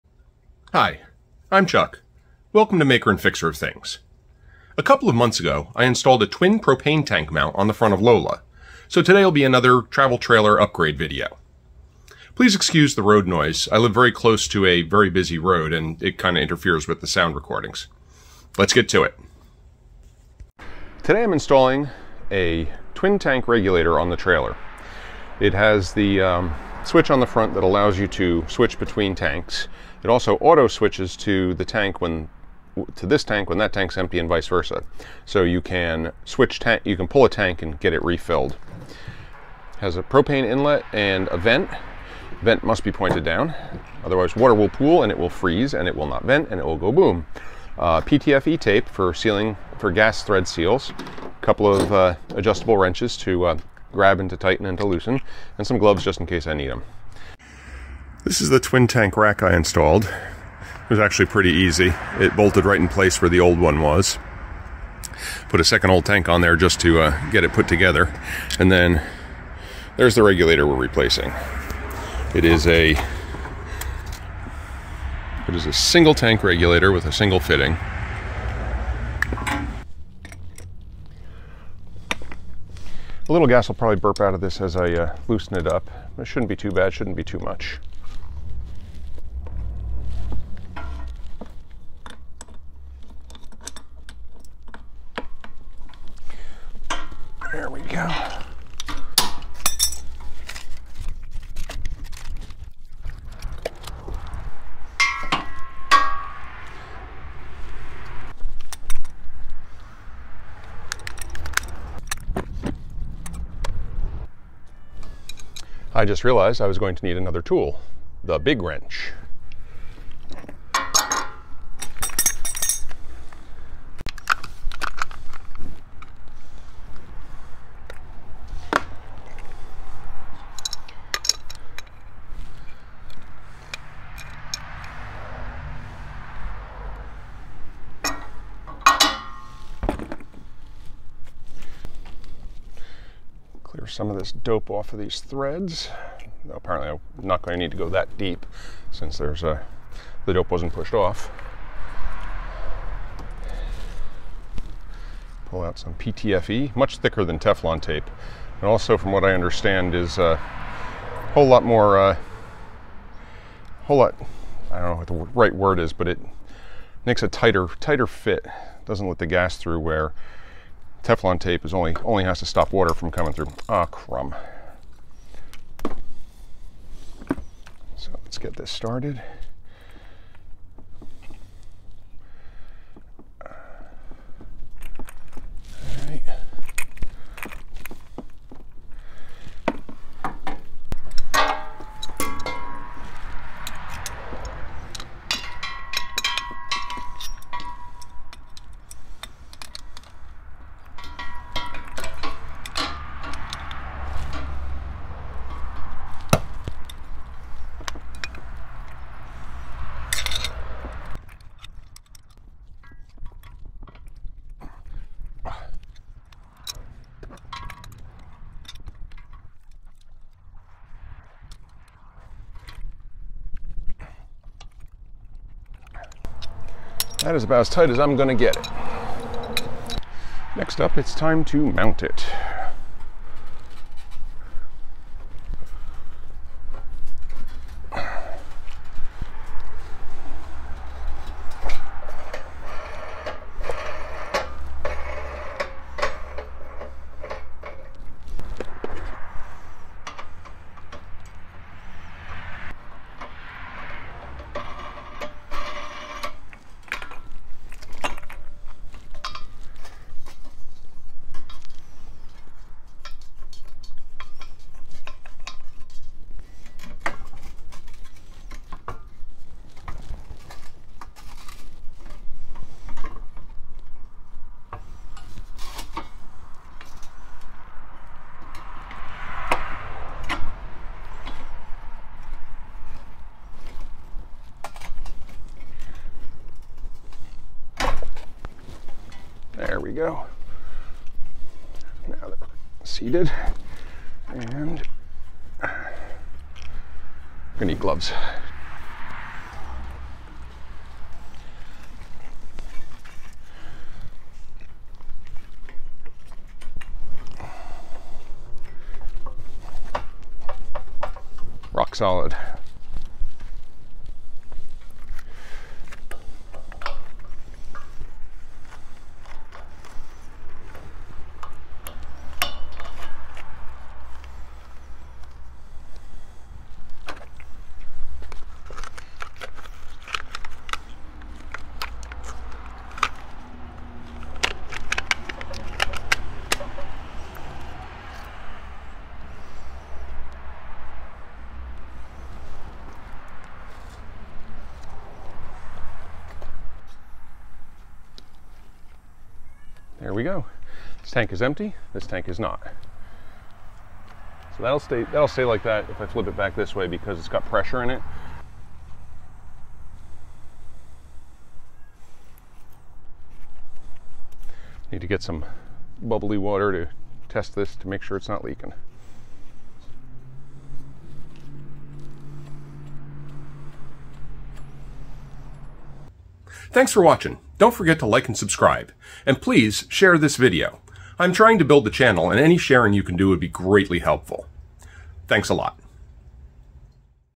Twin Propane Regulator Install!
Upgrading Lola with a twin propane regulator!